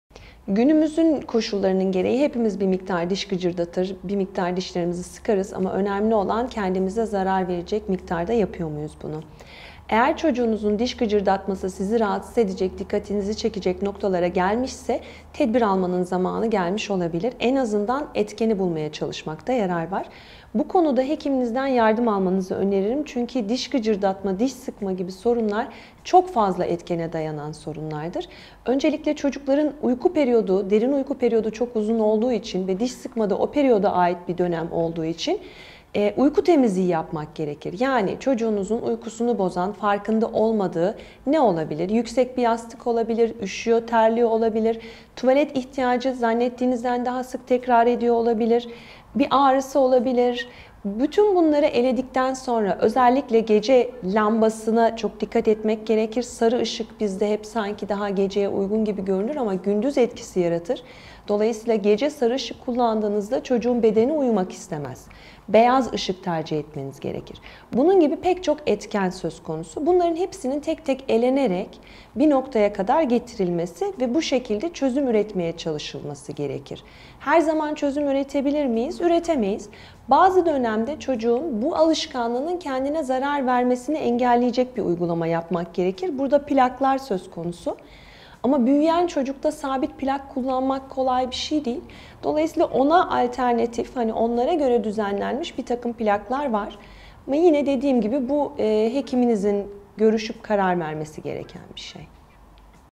Diş gıcırdatmala ve diş sıkmalarında (bruksizm) önlenmesinde neler yapılabilir?
Uzm. Dr. Dt. Şule ŞİMŞEK